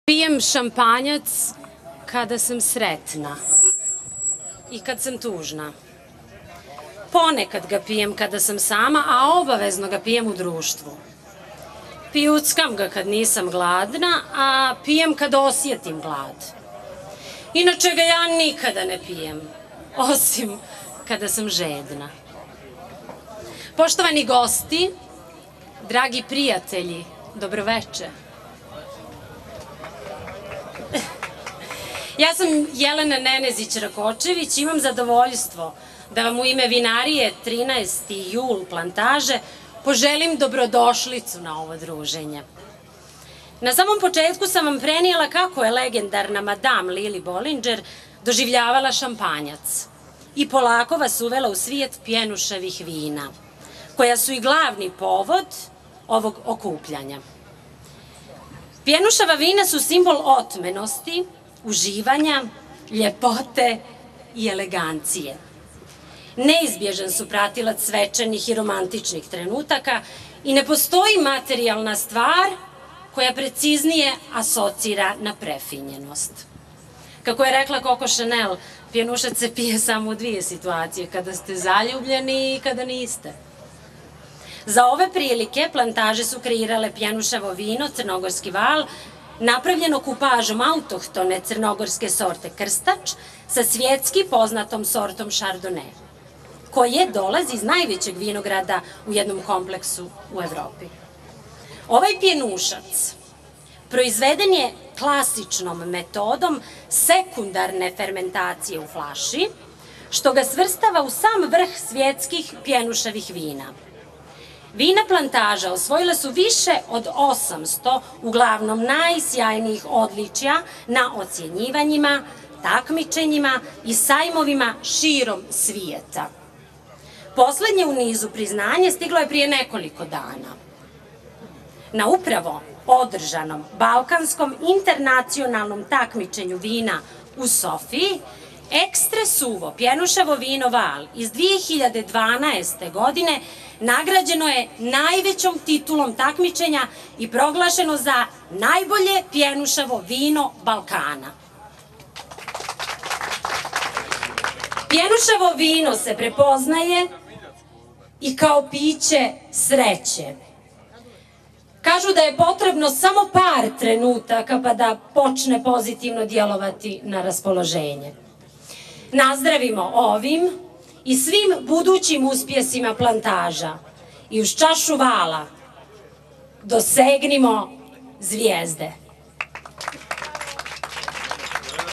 Tivat Glumica Jelena Nenezic Rakocevic na degustaciji najboljeg pjenusavog vina Balkana Crnogorskog
''Crnogorski Val'', prije desetak dana, dobio je nagradu kao najbolje pjenušavo vino Balkana. Tim povodom kompanija ''Plantaže 13. jul'' u eksluzivnom ambijentu organizovala je degustaciju njihovog ekskluzivnog proizvoda.Na nedavno održanoj smotri Balkans International Wine Competition 2016, održanoj peti put u Sofiji, “Crnogorski Val” proglašen je za najbolje pjenušavo vino na Balkanu, čime mu je pripala najveća titula takmičenja. I ostala vina kompanije “Plantaže” osvojila su više medalja, čime se nastavlja trend zapaženih uspjeha naših na velikim regionalnim takmičenjima.
“Tu je bilo nagrada za razne vrste vina,naravno najviše nagrada osvojilo je vino “Vranac” po kome smo i poznati,i odmah poslije njega je Vranac „Pro Corde”,ali je ovo jedna od većih nagrada koju je Val osvojio,zapravo najveća,pa je konačno malo skrenuta pažnja sa tih mirnih vina na pjenušava vina koja su u Crnoj Gori nepravedno zapostavljena.”,kazala je Jovana Šanović,PR Plantaža.
Ovo jedinstveno pjenušavo vino, nastaje kao autentična kupaža šardonea i autohtone crnogorske bijele sorte krstač – koja se uzgaja samo u vinogradu na Ćemovskom polju.
“Ovaj pjenušac proizveden je klasičnom metodom sekundarne fermentacije u flaši što ga svrstava u sam vrh svjetskih pjenušavih vina.”,objasnila je naša poznata glumica Jelena Nenezić Rakočević koja je degustirala vino.
Iz Plantaža su poručili da će uskoro imati i pjenušavo roze vino, ali i da
rade I dodatne projekte, te da građani mogu očekivati i još nešto, a da nije vino.